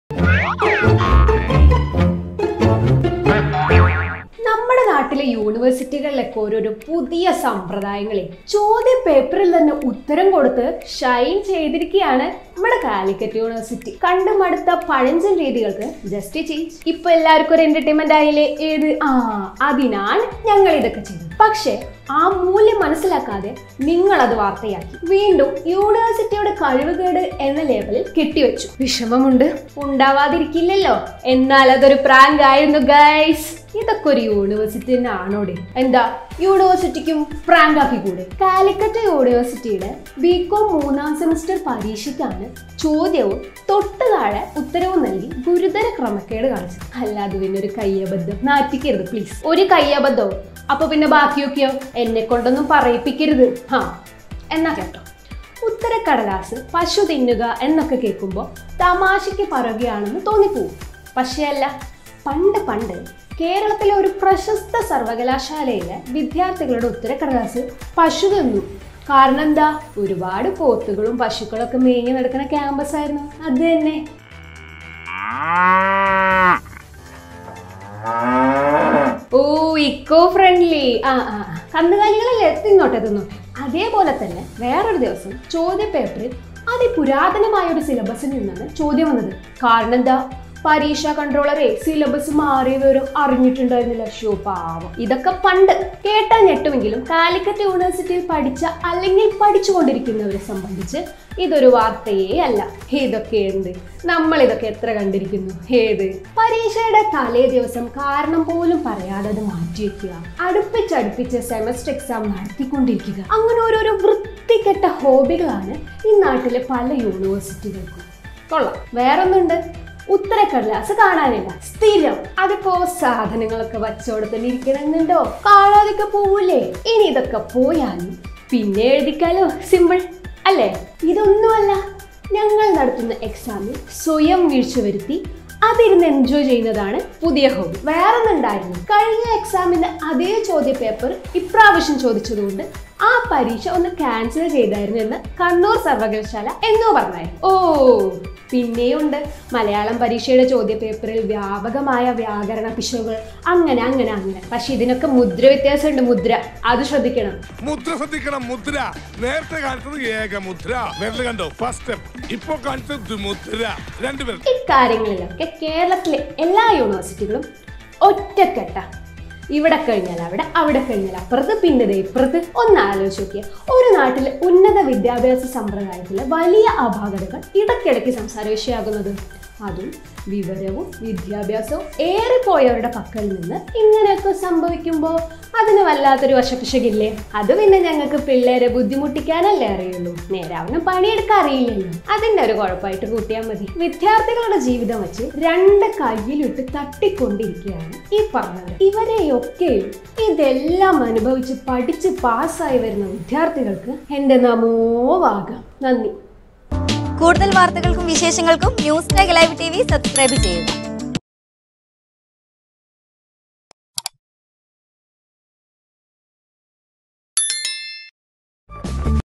യൂ"നിയൻ" വാഴും "സിറ്റി" ചോദ്യം മാത്രമല്ല ഉത്തരവും തരും...Calicut University Exam | Newstaglive TV
ഉത്തരക്കടലാസ് പശു തിന്നുക കേൾക്കുമ്പോളെ തമാശയ്ക്ക് പറയുകയാണ് എന്ന് തോന്നി പോവും. പക്ഷേ അല്ല പണ്ട് പണ്ട് കേരളത്തിലെ ഒരു പ്രശസ്ത സർവകലാശാലയിലെ വിദ്യാർഥികളുടെ ഉത്തരകടലാസ് പശു തിന്നു. കാരണം എന്താ, ഒരുപാട് പശുക്കളും പോത്തുകളും ഒക്കെ മേഞ്ഞു നടന്ന കാംപസ്‌ ആയിരുന്നു.

When you hear the answer sheet eating cow, you will feel that it is telling a joke. But not in the past, the cow ate the answer sheets of the students of a famous university in Kerala. Because it was a campus where a lot of cows and buffaloes grazed.